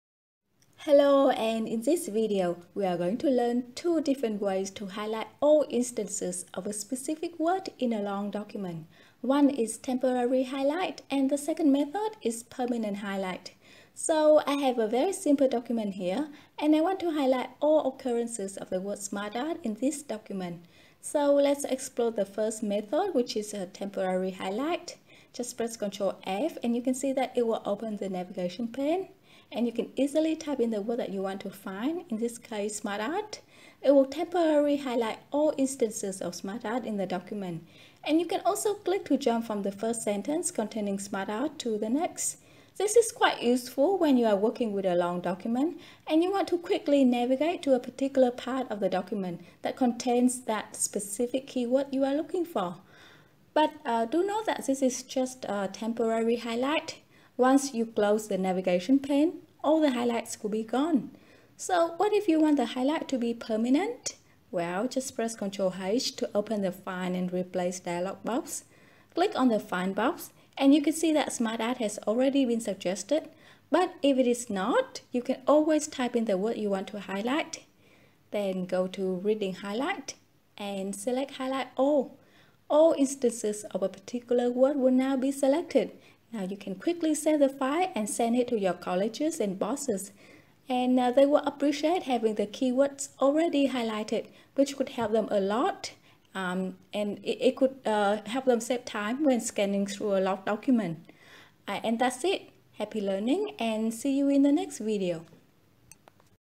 How to highlight all instances of a specific word in a long document
Very often, we would need to find all occurances of a specific keyword, especially in a long document.  In this video, we are going to learn 2 ways to highlight all instances of a particular word throughout a document – temporary highlight and permanent highlight.  We will explore each method in more details  so you can easily decide which one to use when.